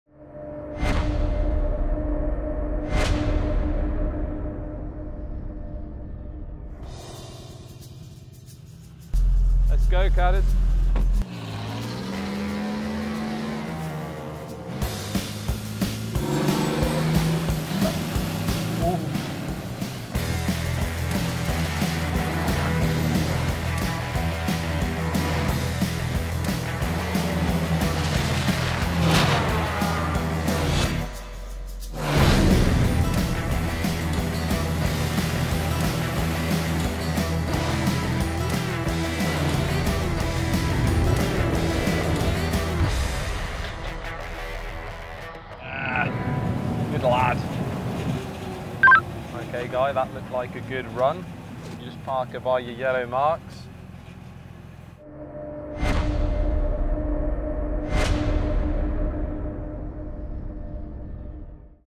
Trailer: Traktor JCB Fastrac 8000 (2019): Rekord - Geschwindigkeit
Zum Ackerpflügen ungeeignet, für Rekorde gemacht – dank eines 7,2 Liter großen Sechszylinders, 1016 PS und 2500 Nm Drehmoment: Der JCB Fastrac 8000 sieht vielleicht aus wie ein Traktor, hat aber mit einem herkömmlichen Ackerpfluggerät so viel gemeinsam wie ein Bugatti Chiron mit einem Lieferwagen. Das Diesel-Biest vom britischen Bau- und Landmaschinenhersteller JCB hat mit seinen vier Monsterrädern, ZF-Sechsgang-Schaltwerk und Aerodynamik-Hilfe vom Williams-Formel-1-Team seinen eigenen Rekord als schnellster Renntraktor der Welt noch einmal verbessert.